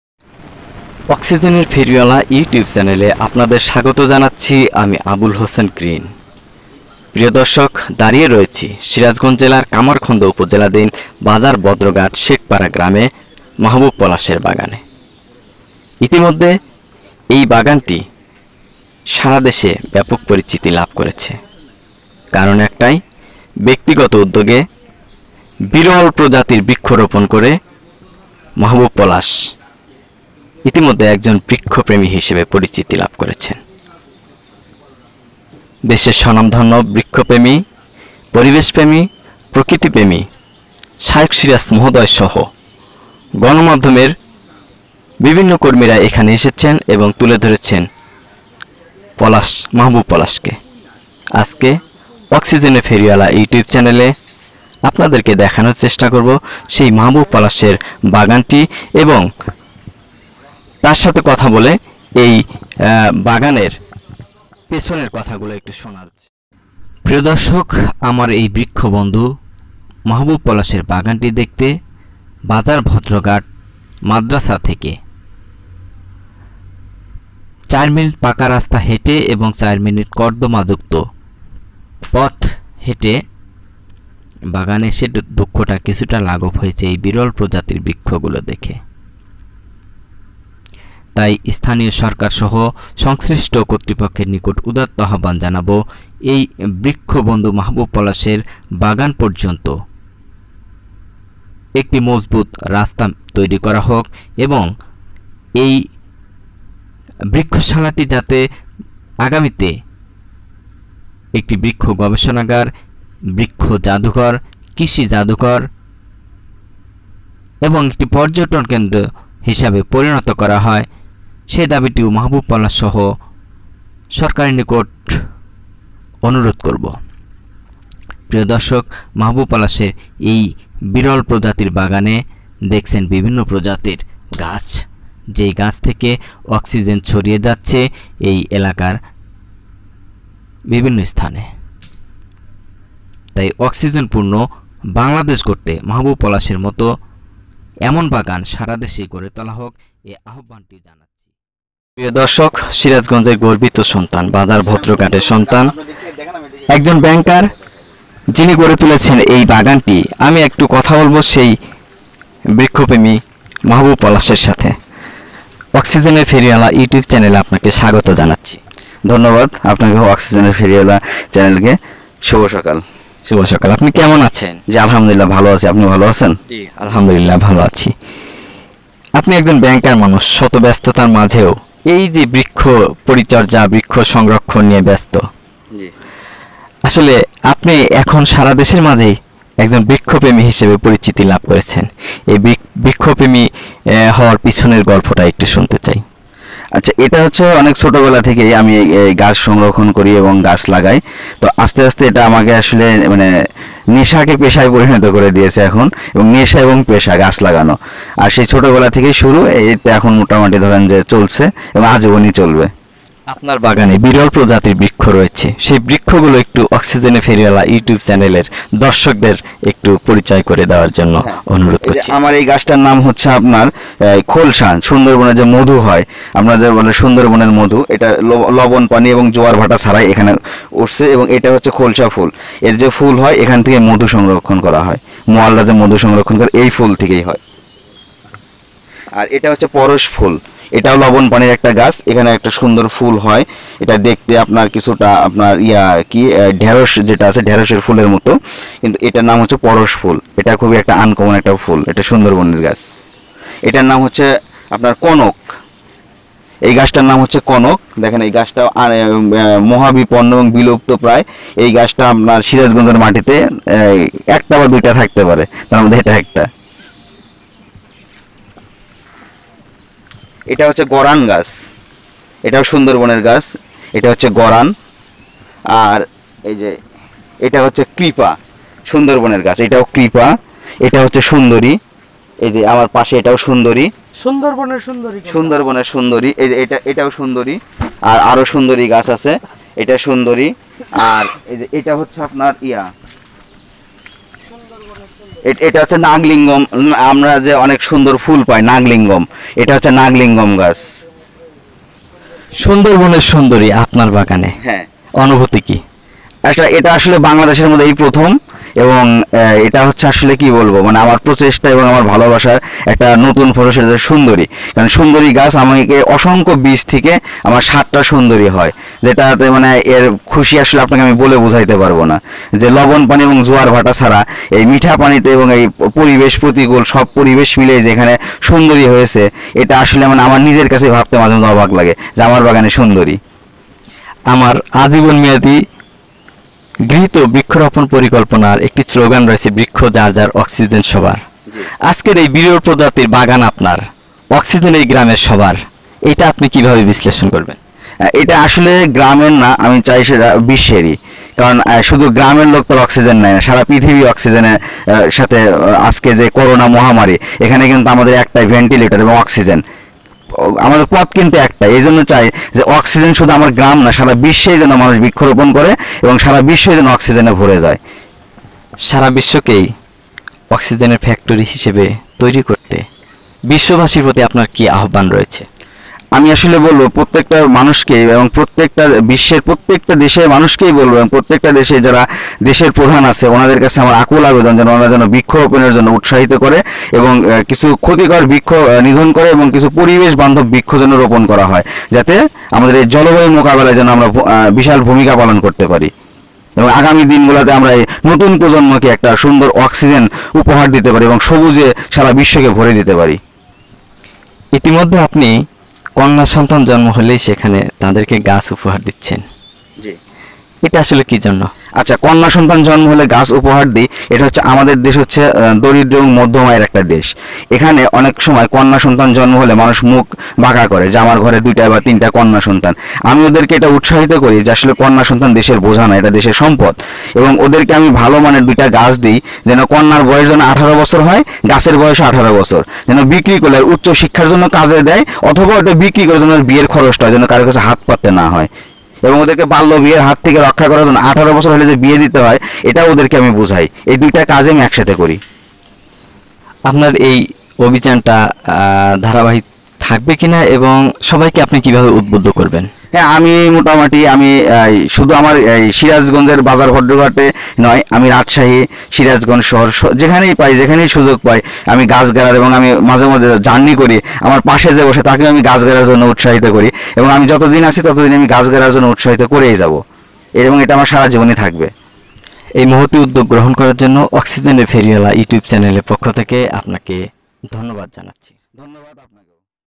৩০-আমার বৃক্ষবন্ধু মাহবুব পলাশ
সিরাজগঞ্জের কামারখন্দ উপজেলাধীন ভদ্রঘাট শেখপাড়ার  আমার বৃক্ষবন্ধু মাহবুব পলাশ তার বাড়ির নিকটে গড়ে তুলেছেন বিরল প্রজাতির বৃক্ষবাগান।অক্সিজেনের ফেরিওয়ালা ইউটিউব চ্যানেলের পক্ষ থেকে মাহবুব পলাশকে অভিনন্দন ও শুভ কামনা জানাচ্ছি।